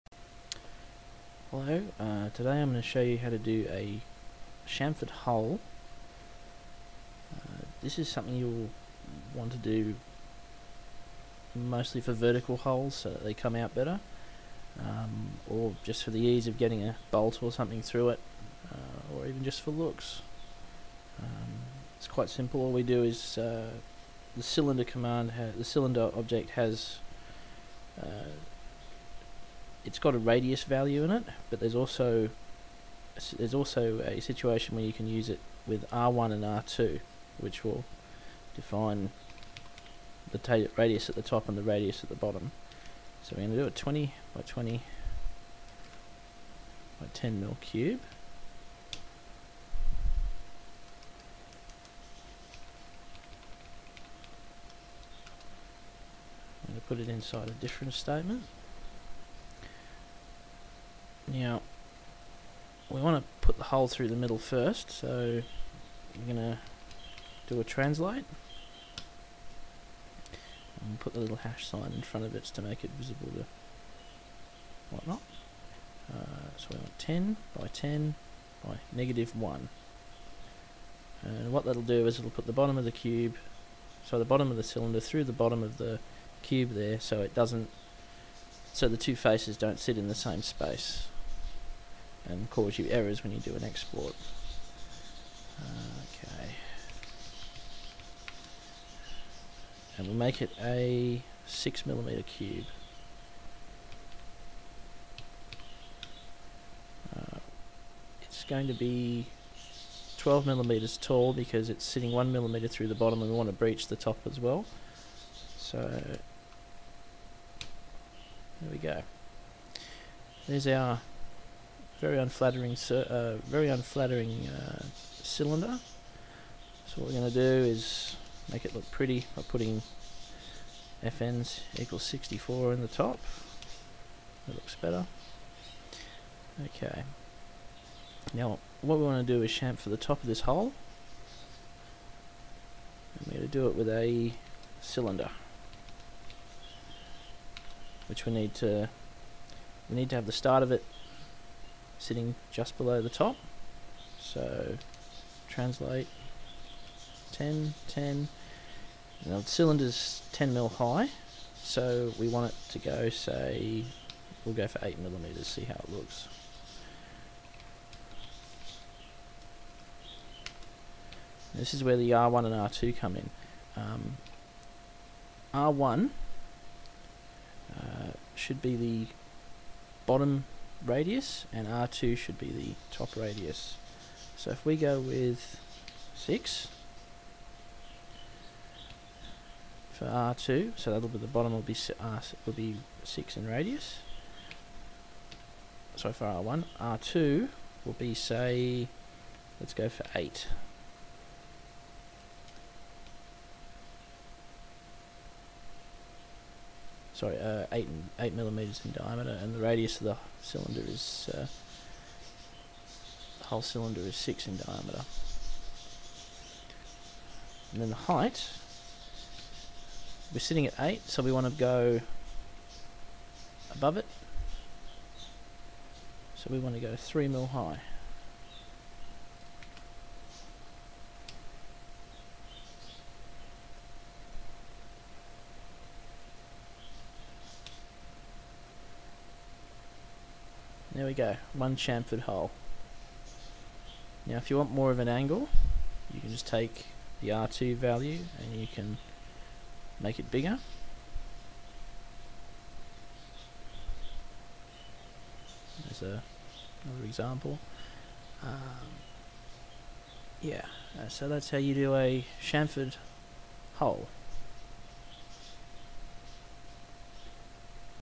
Openscad : how to do a chamfered hole
This is to show how to do a chamfered hole in openscad